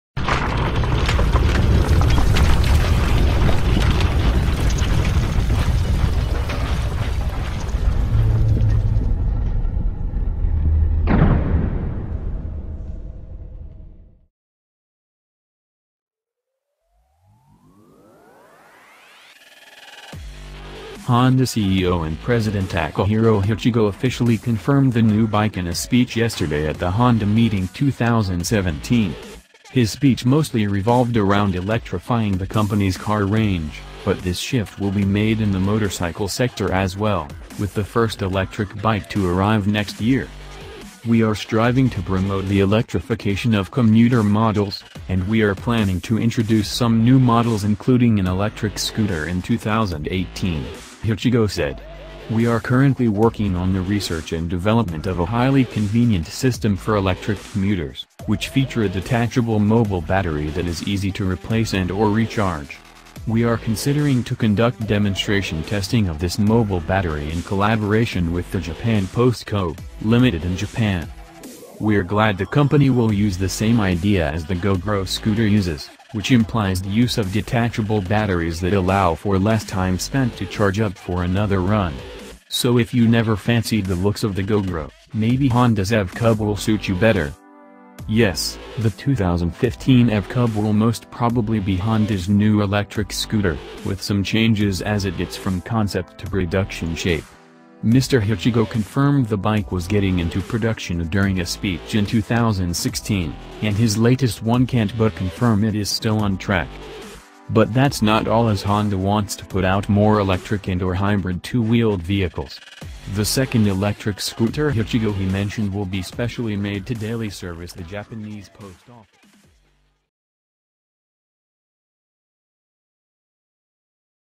LOOK THIS !!!2018 new Honda SH300i Concept Price & Spec
LOOK THIS !!!2018 new Honda SH300i Concept Price & Spec - Honda CEO and President Takahiro Hachigo officially confirmed the new bike in a speech yesterday at the Honda Meeting 2017. His speech mostly revolved around electrifying the company’s car range, but this shift will be made in the motorcycle sector as well, with the first electric bike to arrive next year.

██████ GET DAILY UPDATES! NOW! ██████